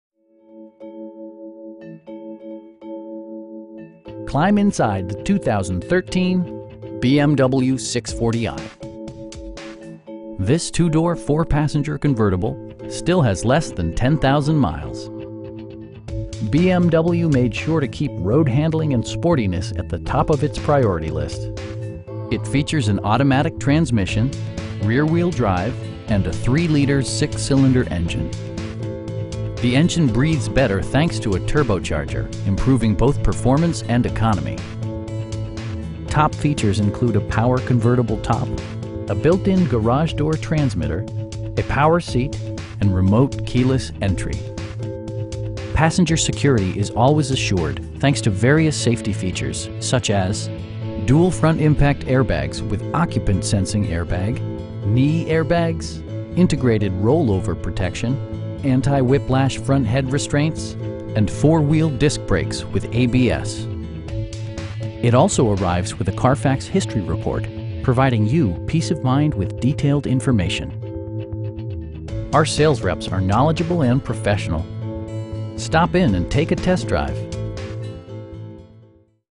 2013 BMW 640i CV F12 in Glendale, CA 91204
Pacific BMW

Climb inside the 2013 BMW 640i. This 2 door, 4 passenger convertible still has less than 10,000 miles! BMW made sure to keep road-handling and sportiness at the top of it's priority list. It features an automatic transmission, rear-wheel drive, and a 3 liter 6 cylinder engine. The engine breathes better thanks to a turbocharger, improving both performance and economy. Top features include a power convertible top, leather upholstery, a built-in garage door transmitter, a power seat, automatic dimming door mirrors, heated seats, rain sensing wipers, and remote keyless entry. With high intensity discharge headlights illuminating your path, you'll always appreciate maximum visibility. Passenger security is always assured thanks to various safety features, such as: dual front impact airbags with occupant sensing airbag, knee airbags, integrated roll-over protection, traction control, anti-whiplash front head restraints, a security system, an emergency communication system, and 4 wheel disc brakes with ABS. Brake assist technology provides extra pressure when applying the brakes. It also arrives with a Carfax history report, providing you peace of mind with detailed information. Our sales reps are knowledgeable and professional. They'll work with you to find the right vehicle at a price you can afford. Stop in and take a test drive!